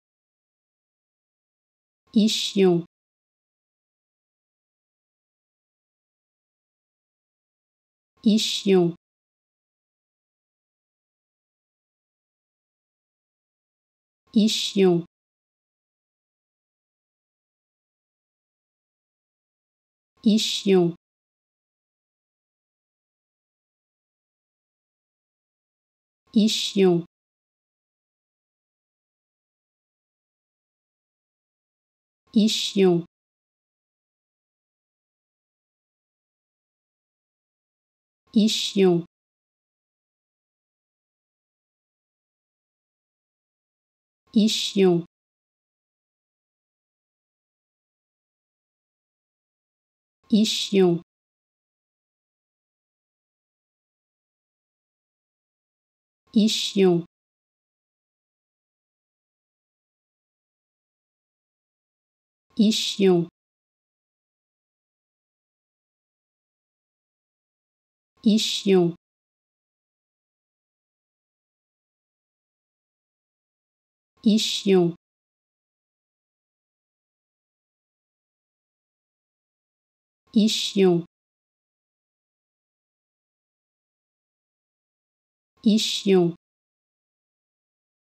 How to Pronounce ''Ischion'' (Ischium) Correctly in French
Learn how to say and properly pronounce ''Ischion'' in French with this free pronunciation tutorial.